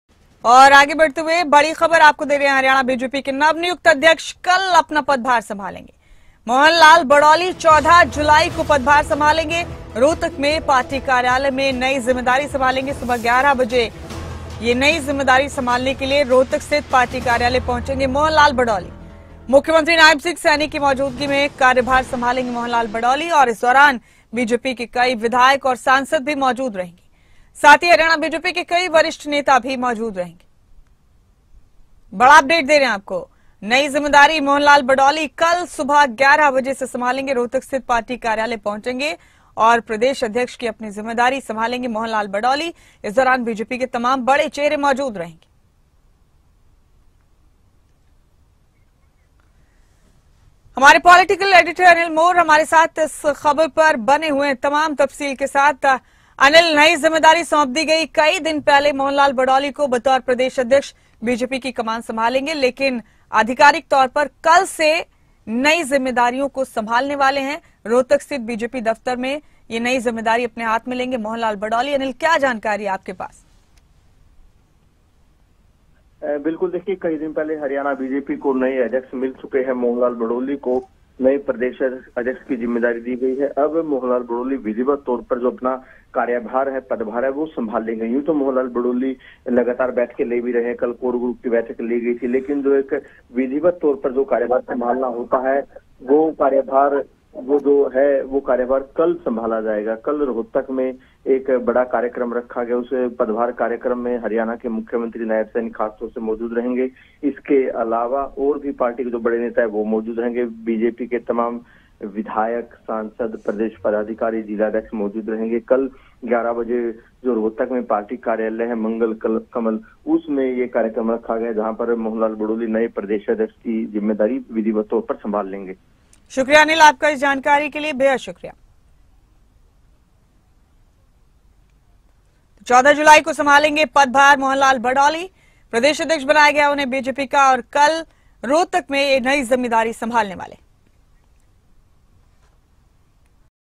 BJP हरियाणा के नवनियुक्त अध्यक्ष मोहन लाल बडौली रोहतक में करेंगे पदभार ग्रहण, 14 को संभालेंगे कुर्सी
Haryana: BJP हरियाणा के नवनियुक्त अध्यक्ष मोहन लाल बडौली रोहतक में करेंगे पदभार ग्रहण, 14 को संभालेंगे कार्यभार